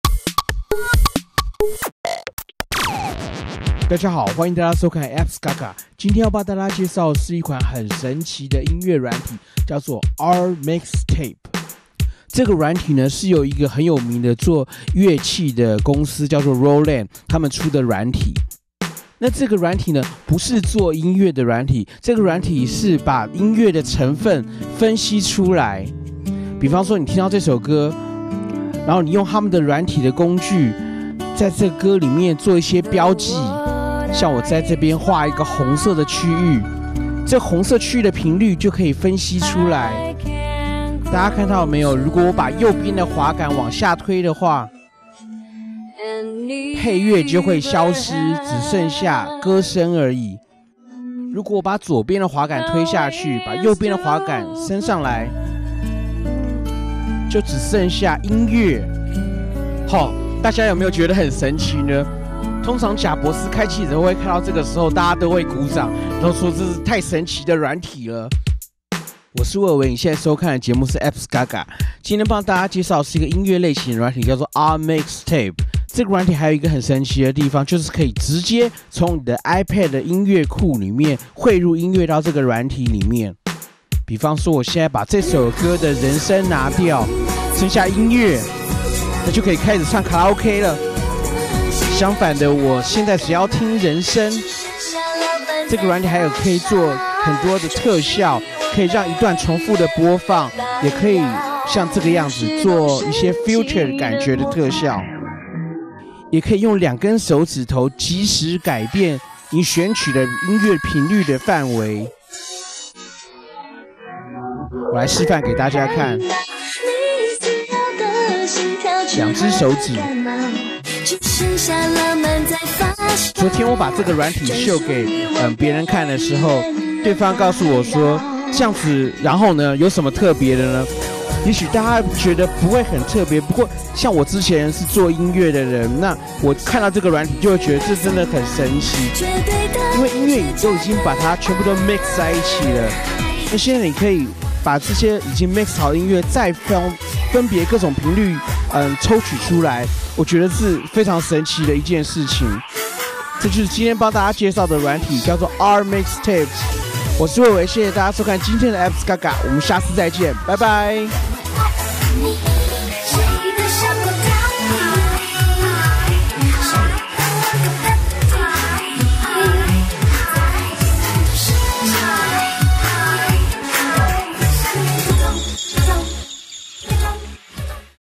[iPad] R-MIX Tab
R-MIX Tab是音樂大廠Roland出的iPad軟體。可以把從iPad的音樂資料庫裡面匯入音樂。經由軟體的效果調校，不僅可以把人聲和音樂分開來，還可以對聲音做出效果。可以把沒有人聲的音樂拿來唱卡拉OK，也可以把單純的人聲拿來做為DJ Mix時的Accapella或是remix單曲時使用。我覺得是一款抄神奇的軟體！看我玩玩看吧！